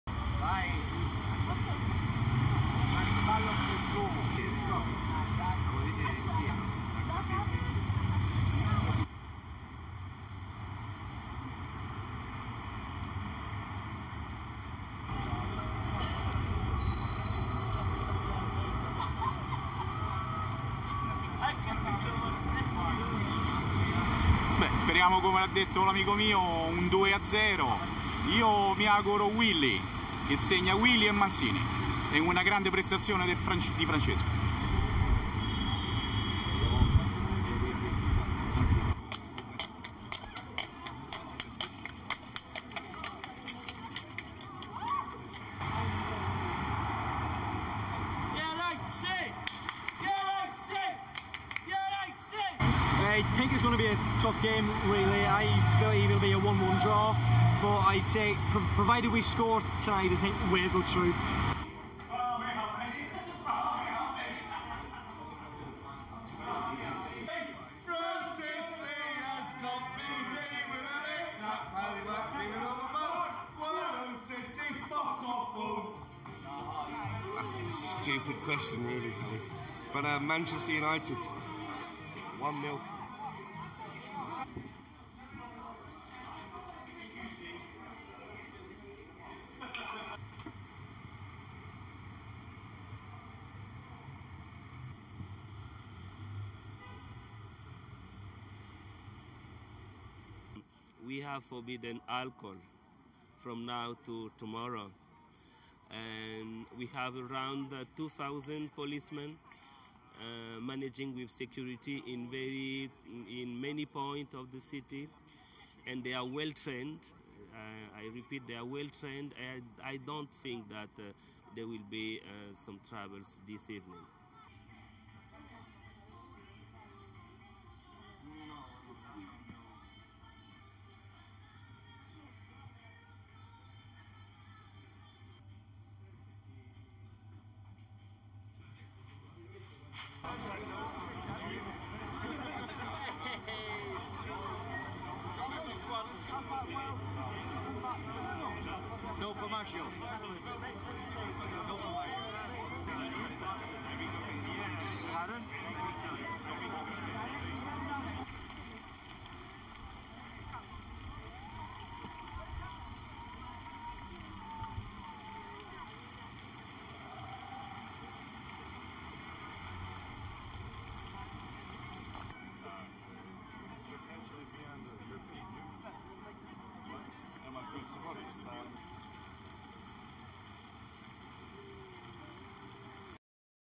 Roma and Man United fans gather for CL's game amongst fears of violence
1. Pan from stand selling T-shirts to wide of Olympic Stadium
2. Close-up of gadgets of Roma and Manchester
3. Closed gates at Olympic stadium
4. Various of souvenir stand
5. SOUNDBITE: (Italian) (no name given), AS Roma supporter:
"As a fried of mine said, let's hope for a two-nil for Roma with scores by (Christian) Wilhelmsson and (Amantino) Mancini and a great performance by Francesco (Totti)."
6. Close-up of T-shirt reading: "Manchester Invasion"
7. Wide of Piazza Navona in Rome as horse drawn cart goes by
8. Tilt down exteriors of pub with supporters of Manchester United shouting: "United, United!"
9. SOUNDBITE: (English) Iain (no surname given), Manchester United supporter:
"I think it is going to be a tough game. I believe it will be a one-one draw, but I think provided that we score tonight, we are going to go through."
10. Interior of pub where Manchester United supporters are gathering
11. Manchester United supporters singing
12. SOUNDBITE: (English): Jason, Manchester United supporter:
Q: "who is going to win tonight?"
"Stupid question really, Manchester United one-nil".
13. Wide of pub with supporters at bar
14. Tilt down warning from the Rome municipality
15. Warning reading: "It is forbidden the sales of alcoholic drinks".
16. SOUNDBITE: (English) Jean Leonard Touadi, Security Councillor for Rome:
"We have forbidden alcohol from now to tomorrow, and we have around 2-thousand policemen managing security in many points of the city, and they are well-trained, I repeat, that they are well-trained and I don't think that there will be some troubles this evening."
17. Various of shelves with alcoholic drinks with warnings inside supermarket
18. Various of exteriors of pub with Manchester United fans drinking
19. Sign on pub door reading: "No drinks outside between 12pm and 3 am.  By order of Rome City Council
20. Various of Piazza Navona
21. Various of police officers at Piazza Navona
STORYLINE:
There was little sign of trouble in the Italian capital of Rome on Wednesday, ahead of the Champions League quarterfinal clash between Manchester United and AS Roma.
The two teams face off on Wednesday night at the Olympic Stadium for the first leg of their tie and according to the Italian newspaper 'Corriere della Sera', over 5-thousand Manchester United fans have travelled to Italy for the game.
Security is a big issue on the streets of Rome ahead of the match.
Officials from the city of Rome and Manchester United met after the English football club posted a warning on its website warning fans to beware of the hardcore "ultras" element of Roma's support.
Supporters of the Red Devils were also warned against using the city's underground network to reach the Olympic Stadium.
The Italian city's mayor reacted strongly to the warnings, saying the team was creating a "negative climate."
But there was no evidence of a 'negative climate' early on Wednesday as supporters of both teams prepared for the big match.
And naturally supporters of each team maintained their allegiances.
"Let's hope for a two-nil for Roma with scores by Wilhelmsson and Mancini and a great performance by Francesco (Totti)," one Roma supporter told AP Television.
But Red Devils fan gave the win to "Manchester United one-nil".
But Iain, a Manchester United supporter, thought it would be a tough game for both sides.
"I believe it will be a one-one draw, but I think that, provided that we score tonight, we are going to go through" he said.
The visiting fans were enjoying themselves in the drinking establishments in Rome on Wednesday.